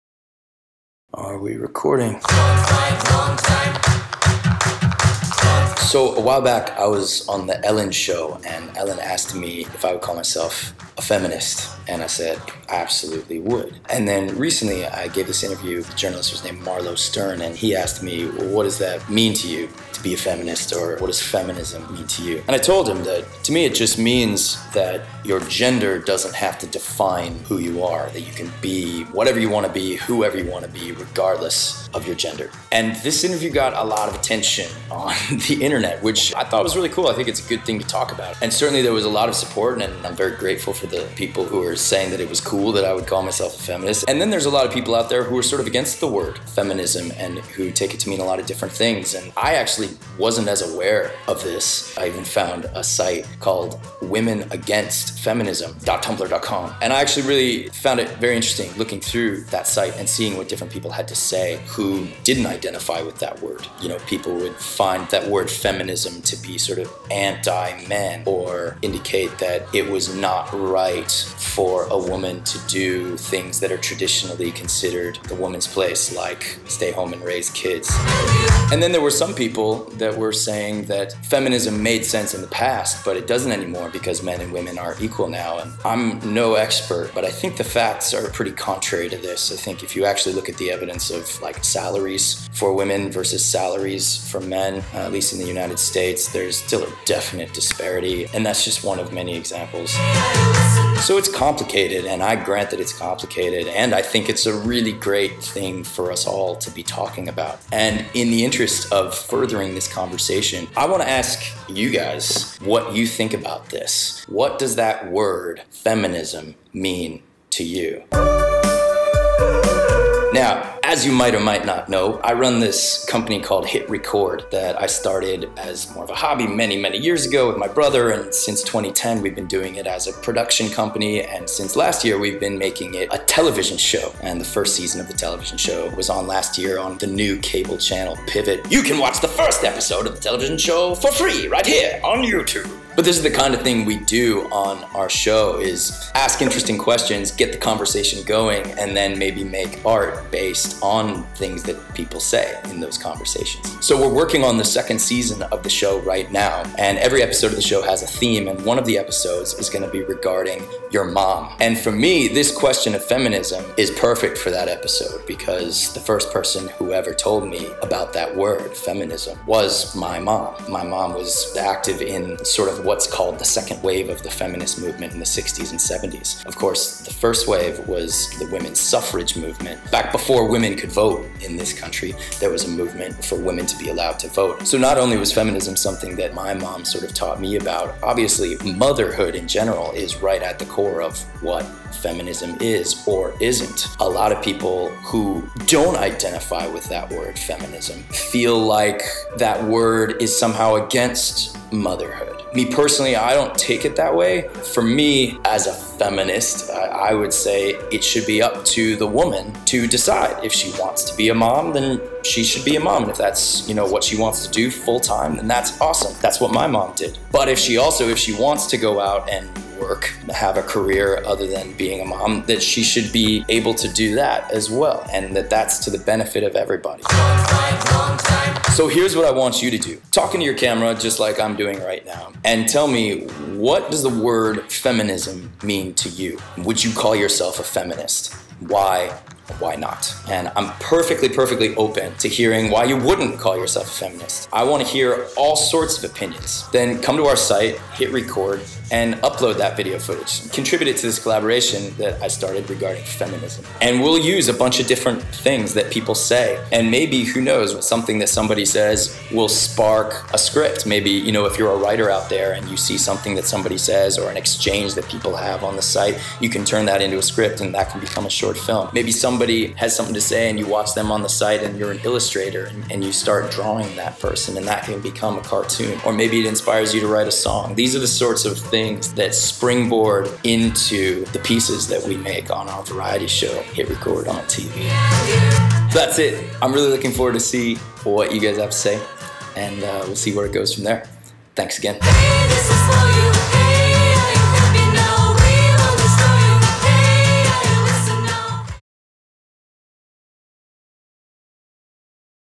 RE: Feminism (REQUEST)
EVERYONE: RECord yourself on Video answering the following questions:
- What does the word "Feminism" mean to you?
- Are you a Feminist? Why or why not?
Thanks!